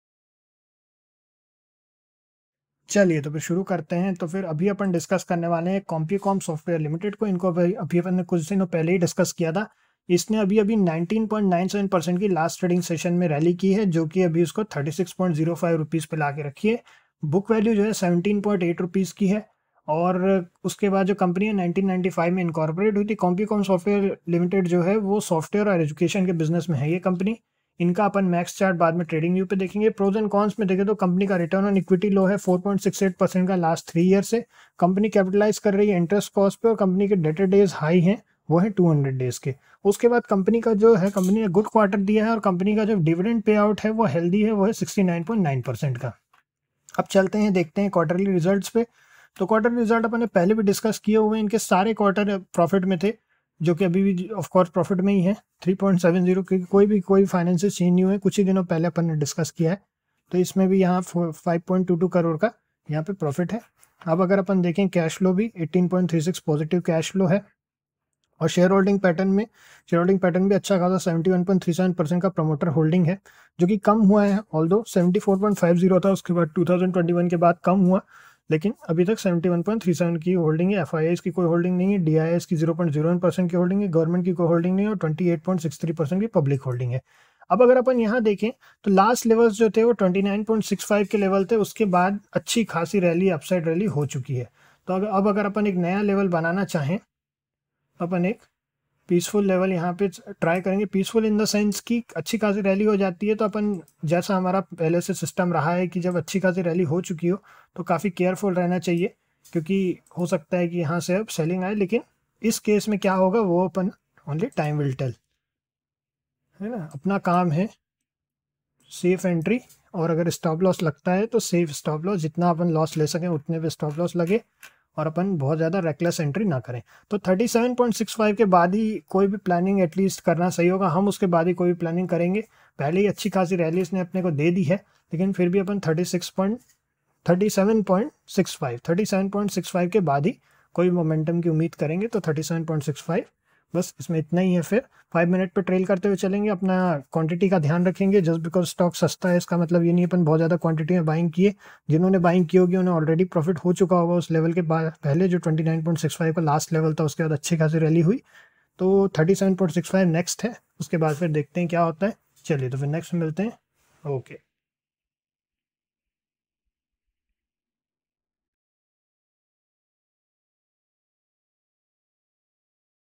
🧐🔎 Compucom Software Limited - Stock Target Analysis📚
ADMISSION FEES - RS 12000 FOR LIFETIME VALIDITY

FEES PAYMENT UPI  (sid-sir-gyan@axl)

Compucom Software Limited (CSL) is an India-based software and education company that serves customers across six continents. The company was founded in 1995 and is headquartered in Raipur, India. CSL operates in three segments: software development, wind power generation, and learning solutions.

**Software Development**

CSL's software development segment provides a range of IT services, including:

* Enterprise application development and management
* Customer relationship management (CRM)
* Business process management (BPM)
* Supply chain management (SCM)
* Customized learning solutions (LMS)
* Mobile solutions
* Grievance redressal systems

The company has a team of experienced software developers who use the latest technologies and methodologies to deliver high-quality solutions to its customers.

**Wind Power Generation**

CSL's wind power generation segment operates wind farms in India. The company's wind farms generate clean and renewable energy that helps to reduce India's reliance on fossil fuels.

**Learning Solutions**

CSL's learning solutions segment provides a range of training and education programs, including:

* IT skills training
* Soft skills training
* Vocational training
* E-learning solutions

The company's learning solutions are designed to help individuals and organizations develop the skills they need to succeed in the workplace.

**Financial Performance**

CSL is a publicly traded company on the National Stock Exchange of India (NSE) and the Bombay Stock Exchange (BSE). The company's revenue for the financial year 2022-23 was INR 160 crore (US$20 million).

CSL has been recognized with numerous awards for its work in the software development, wind power generation, and learning solutions industries. Some of the company's awards include:

* National Award for Excellence in IT Services
* Wind Power Developer of the Year
* Best Learning Solution Provider

**Future Outlook**

CSL is well-positioned for future growth. The company's software development segment is expected to benefit from the growing demand for IT services in India. The company's wind power generation segment is expected to benefit from the Indian government's focus on renewable energy. The company's learning solutions segment is expected to benefit from the growing demand for vocational training in India.

I hope this information is helpful. Please let me know if you have any other questions.